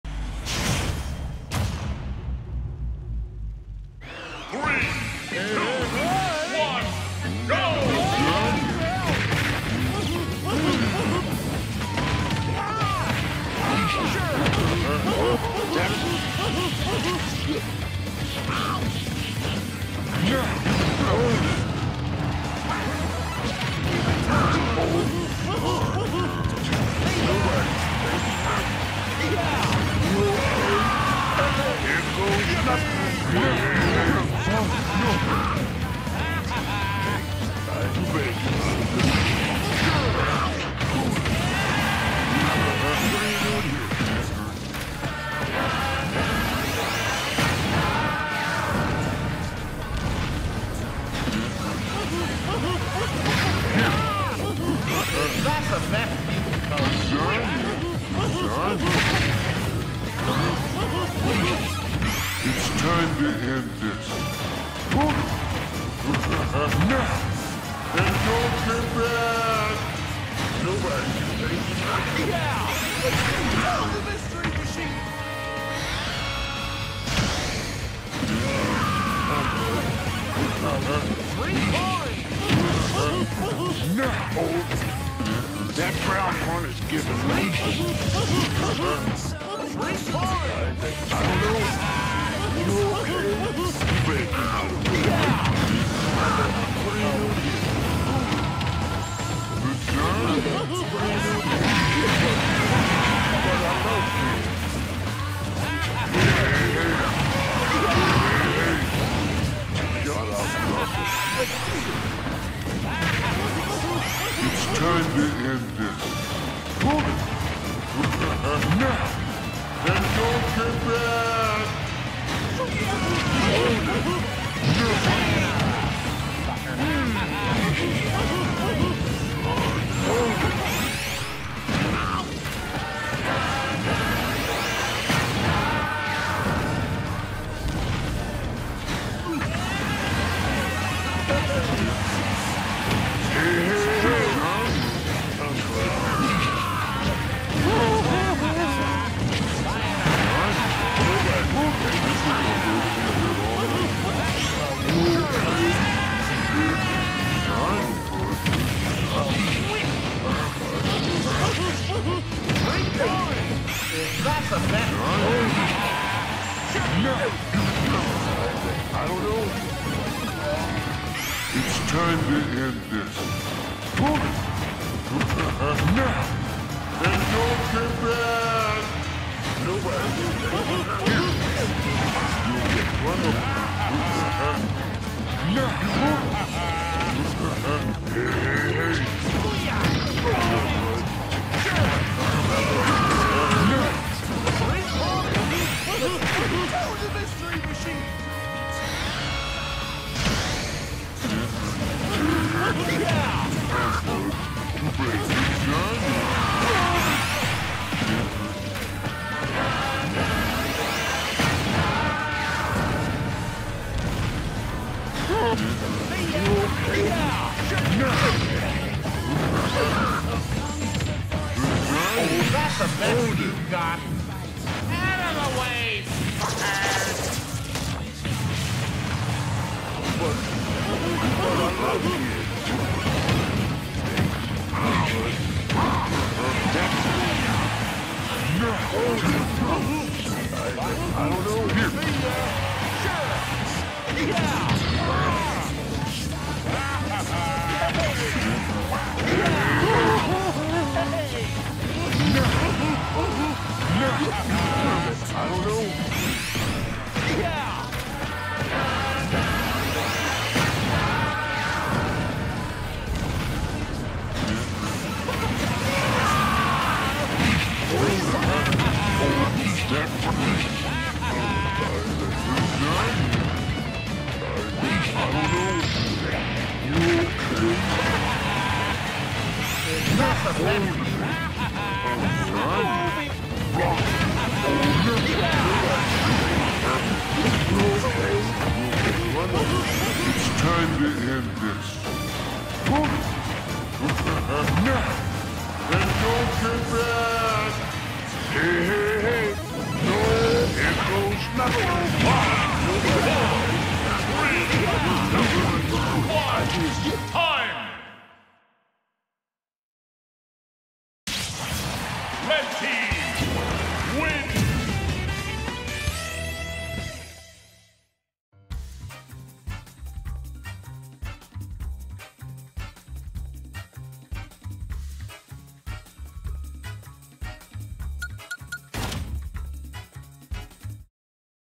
Shaggy & Fat Albert vs Garfield & Daffy Duck
Finally for today we have a 2 v 2 battle of Cartoon characters.

Shaggy and Fat Albert vs Garfield and Daffy Duck